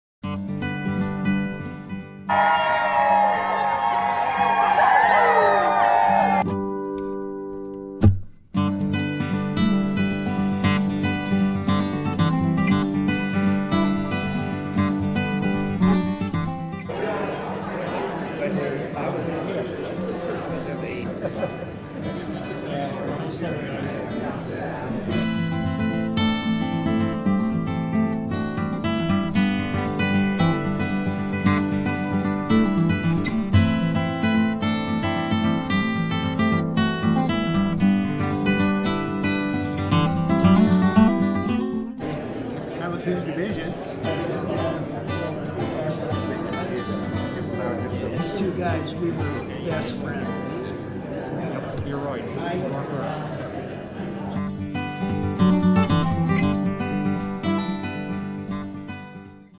Students Welcomed Veterans at Stivers School for the Arts
Students at Stivers School for the Arts welcomed veterans to their school on Valentine's Day. The "We Love Our Vets" Valentine's Day program gave DPS students an experience to remember. Thanks to all the vets who shared their wisdom with our students!